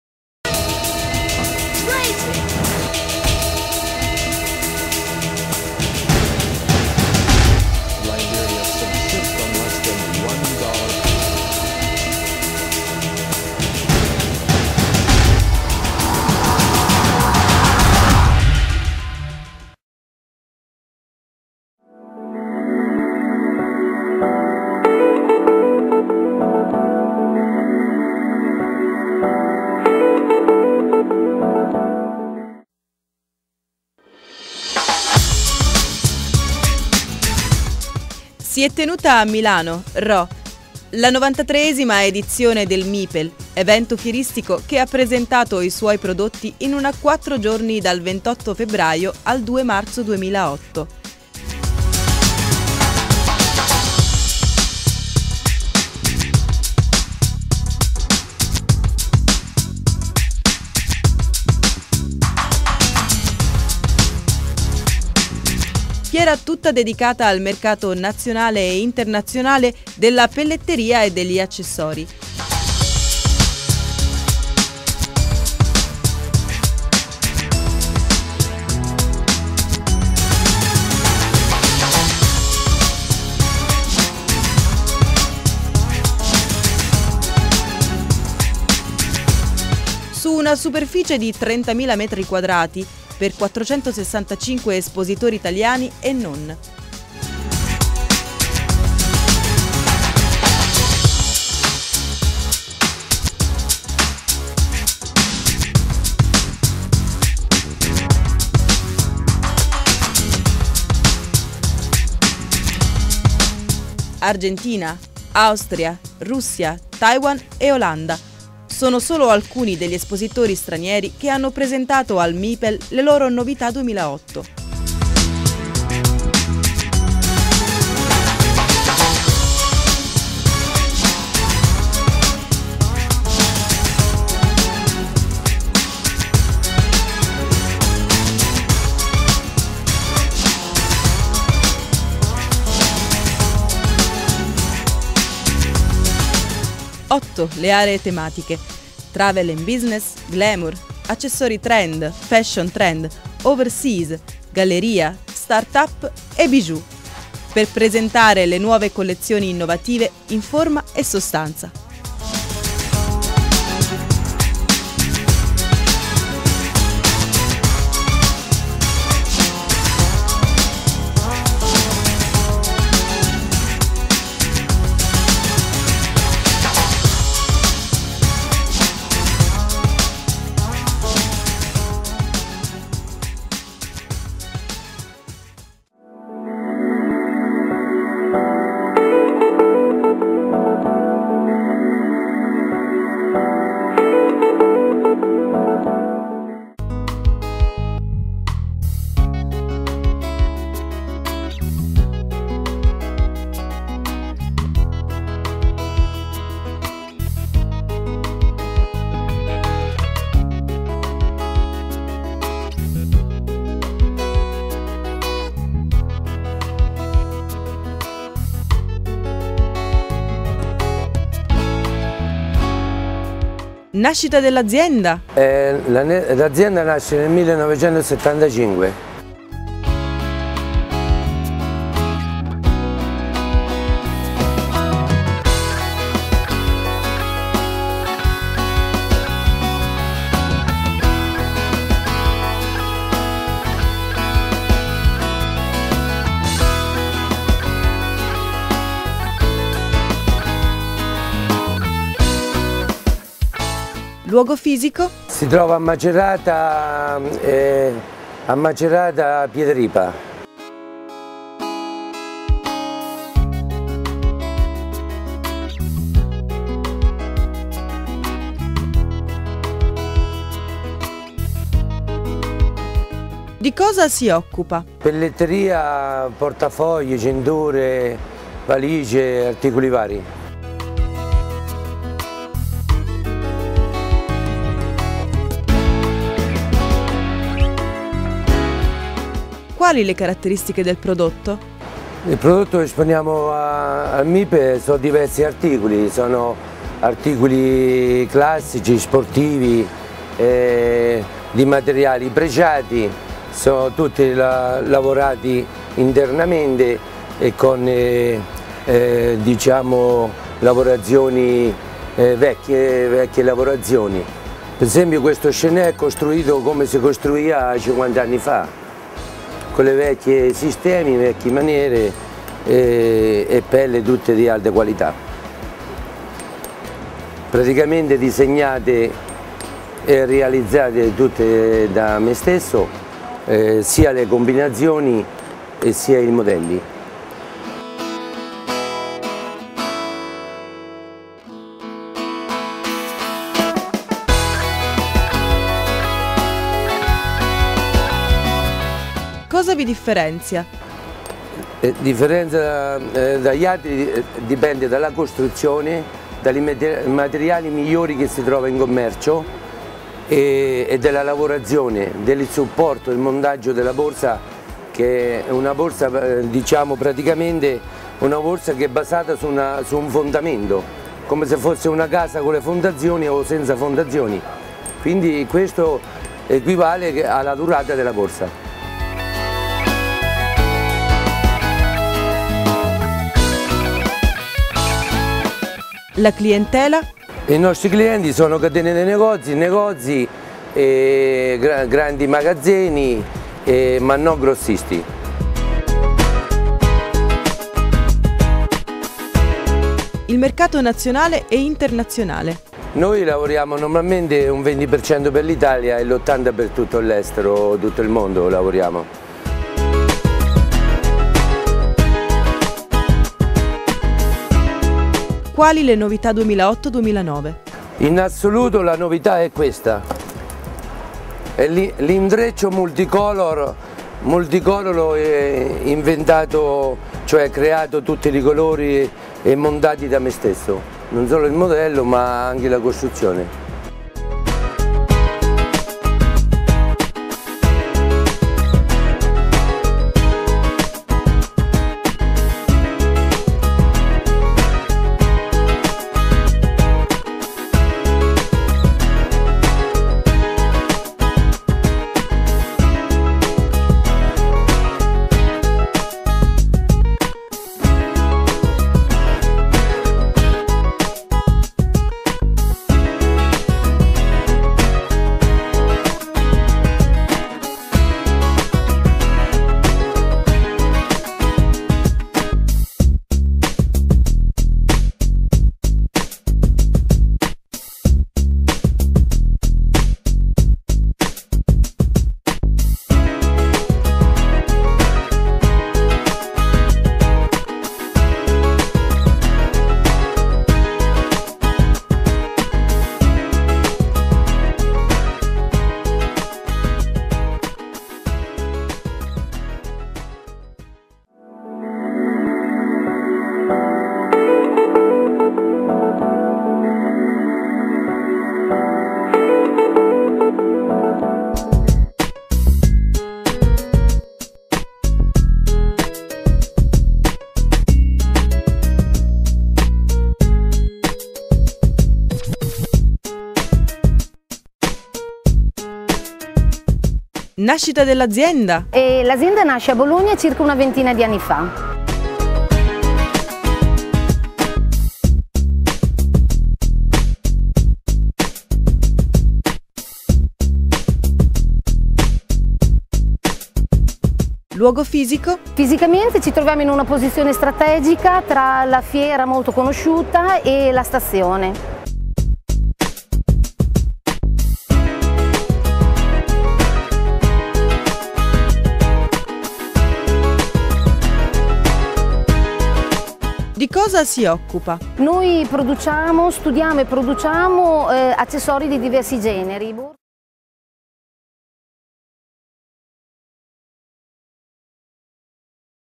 Store Attualità_speciale Eventi - MIPEL 2008
Da Milano l'importantissimo evento internazionale dedicato alla pelletteria